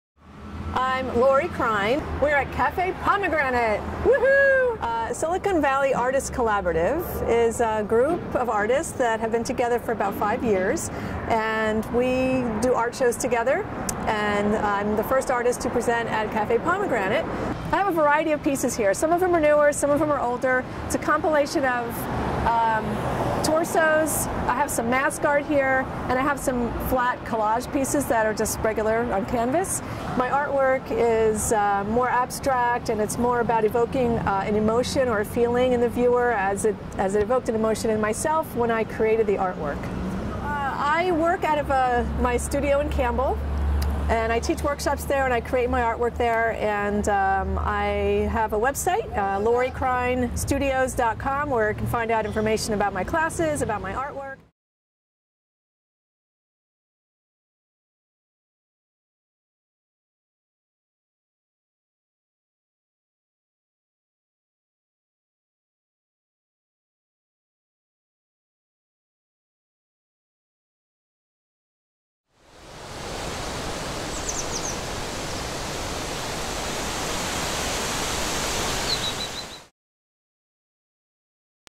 Art @ the Cafe - Seduction - Lori Krein
Lori Krein's art show Seduction, the first in the Art @ the Cafe series, a collaboration between Silicon Valley Artists Collaborative and Café Pomegranate.

Seduction runs through March, 2014

Artist's reception March 13, 6 PM to 9 PM
Café Pomegranate